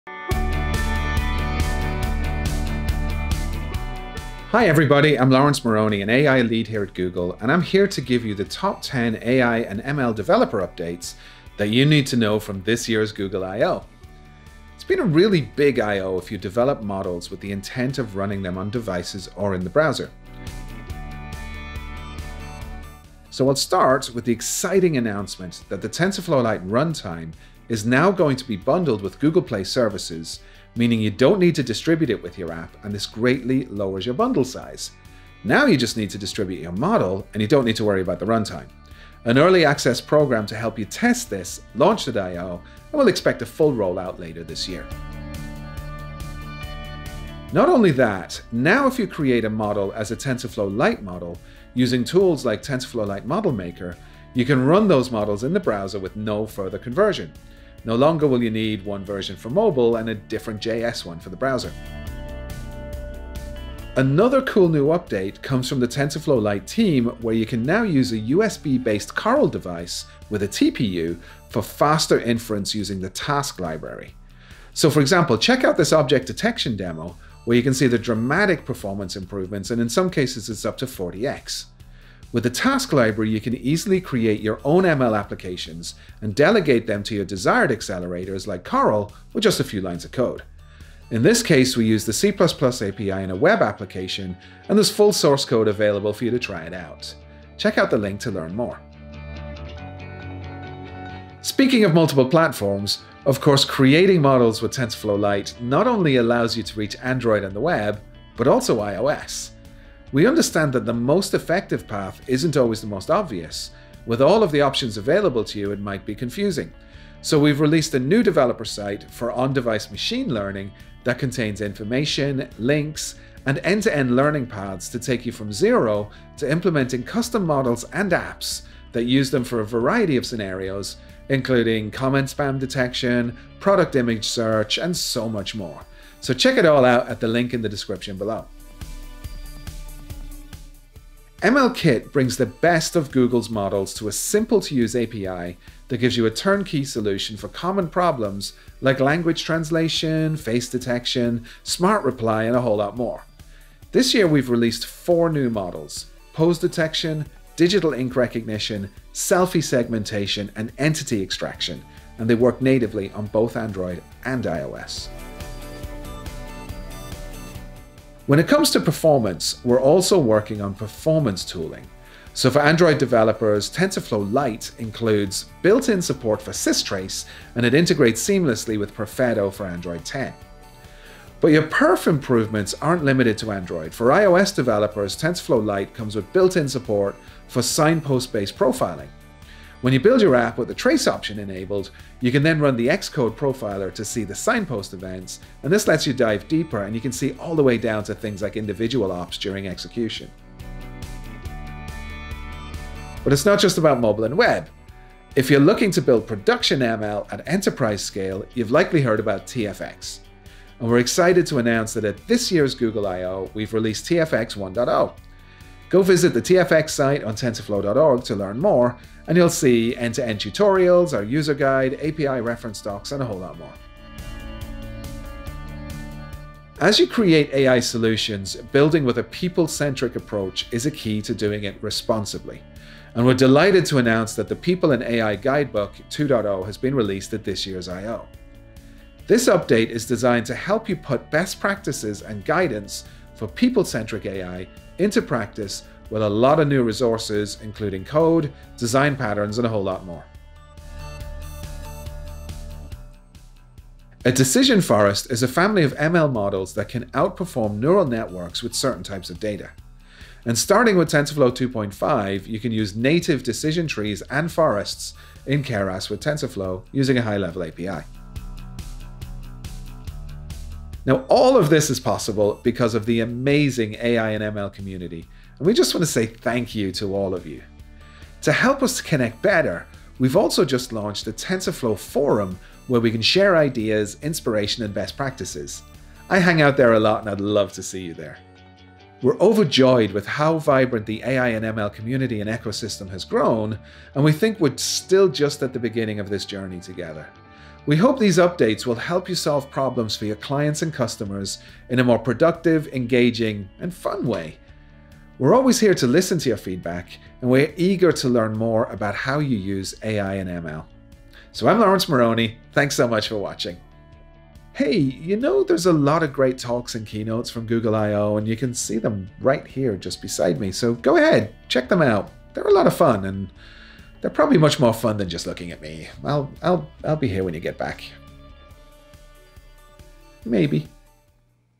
Top 10 AI and ML developer updates from Google I/O 2021
In this video, AI Lead Laurence Moroney gives us the top 10 AI and ML developer updates from this year’s Google I/O. We’re excited with how much the AI and ML ecosystems have grown, and we hope these updates will help you solve any future challenges you face.

00:00 Introduction
00:29 - #1 Reduce your app size with TensorFlow Lite
00:45 - #2 Run TensorFlow Lite Models on the web
01:02 - #3 Accelerated TPU inference using the Task library
01:40 - #4 Learn about On-Device Machine Learning on Google Developers
02:19 - #5 Update to ML Kit
02:46 - #6 Android & iOS profilers
03:22 - #7 Announcing TFX 1.0
03:45 - #8 Announcing People + AI Guidebook 2.0
04:11 - #9 Decisions Decisions - Learn about Decision Trees
04:29 - #10 Check out the new TensorFlow Forum
04:49 - Wrap up

product: TensorFlow - General; event: Google I/O 2021; fullname: Laurence Moroney; re_ty: Publish;